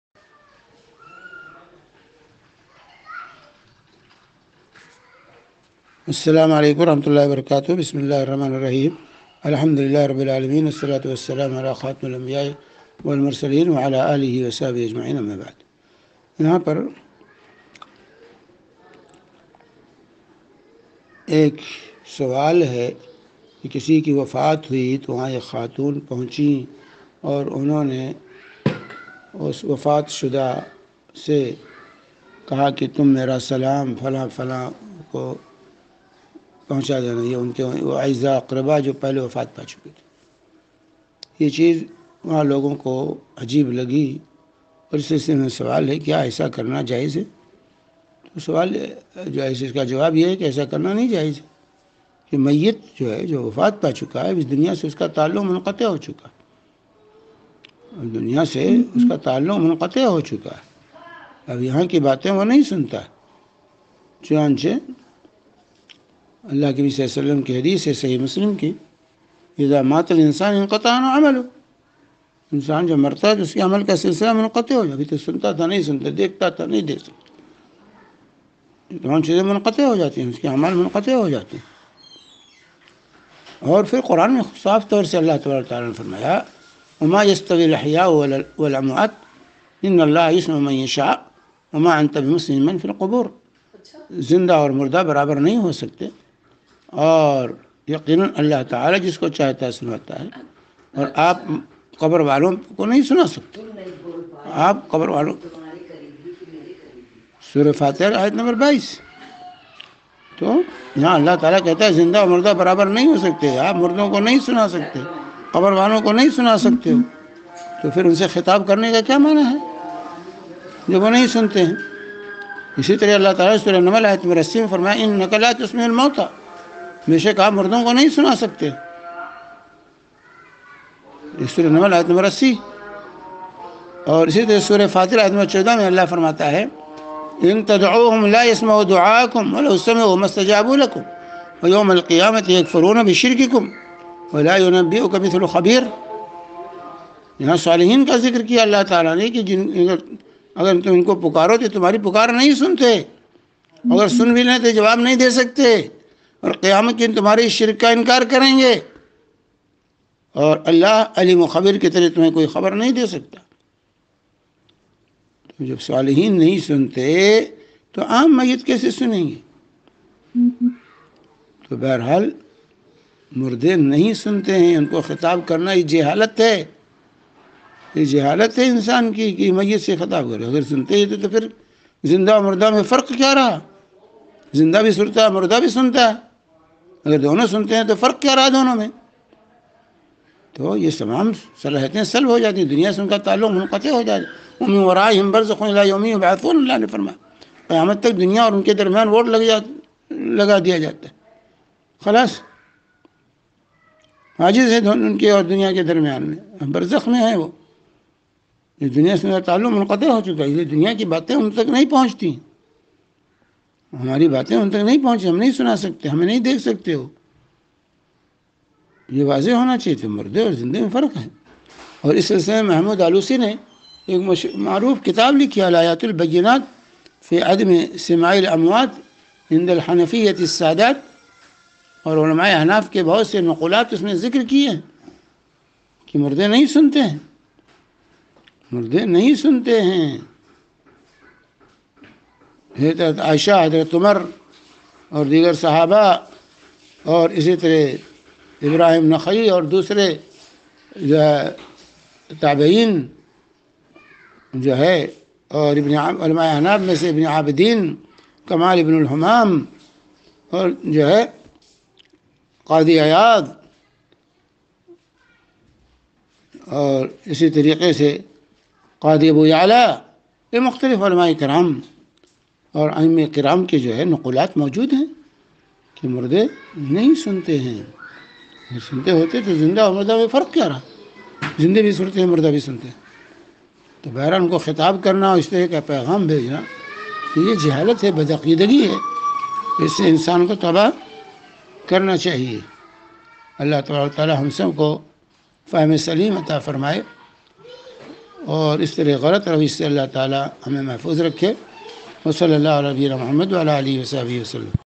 Murday ky pass kharay ho kar us ko kehna ky falan ko salam pohncha dain yh jaiz hai?
فضیلة الشیخ شمیم سلفی ۔قطر